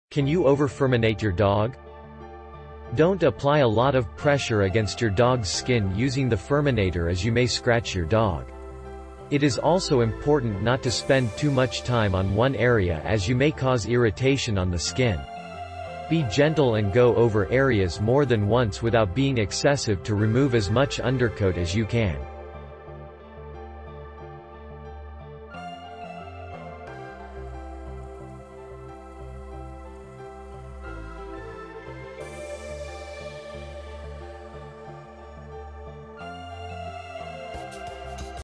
Can you over Furminate your dog?
Don't apply a lot of pressure against your dog's skin using the FURminator as you may scratch your dog. It is also important not to spend too much time on one area as you may cause irritation on the skin. Be gentle and go over areas more than once without being excessive to remove as much undercoat as you can.

Reference:
How to Brush a Dog With FURminator - Wag!